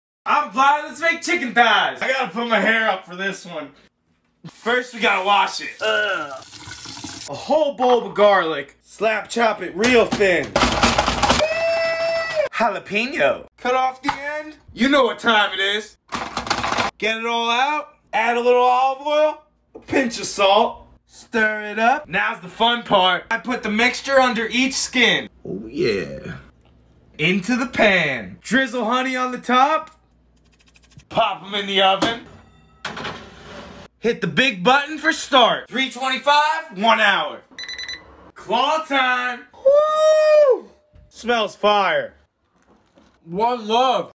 How to make sweet and spicy chicken thighs
Blind chef Anthony Ferraro shows us his special chicken thigh recipe